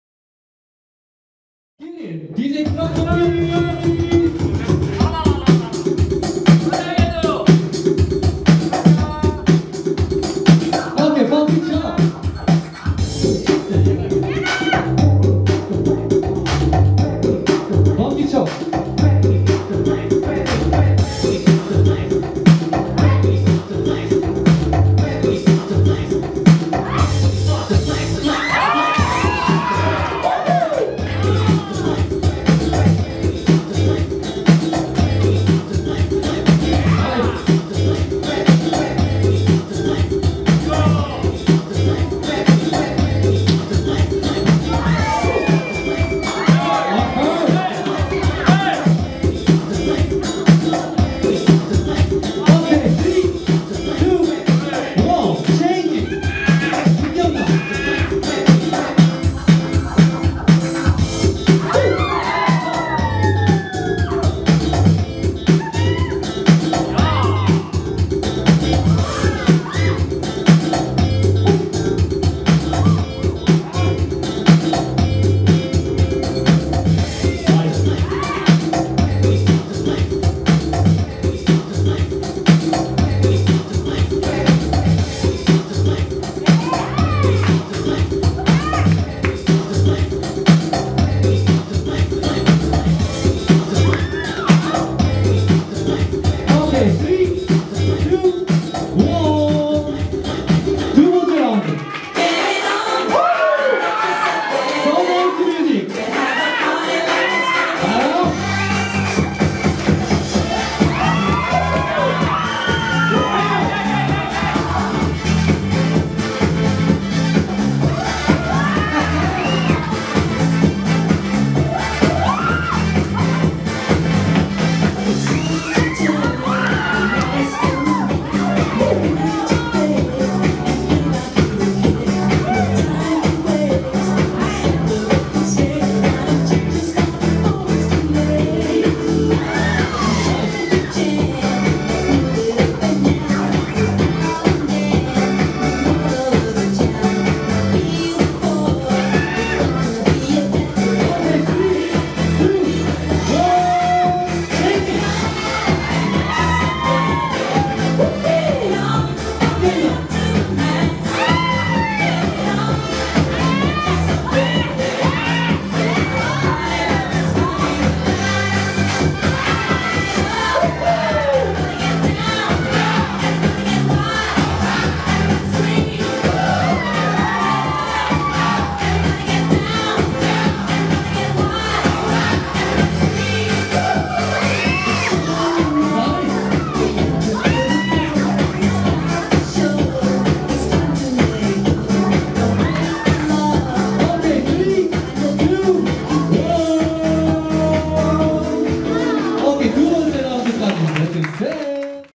8강 이해동 vs 김경남 THEPLAYER0627
더플레이어 미니배틀0627

-JUDGE : SOLSOW
-DJ : BART
-MC : DUGONG

-날짜 : 6월 27일 일요일
-시간 : 오후4시 (3시부터 입장가능)
-장소 : 포인트댄스

-참가비 : X
-상금 : X

-WINNER : 상패, IDMB (티셔츠+모자)
-2nd : 상패, IDMB (티셔츠+모자)
-Best4 : IDMB (티셔츠)

present by. POINT DANCE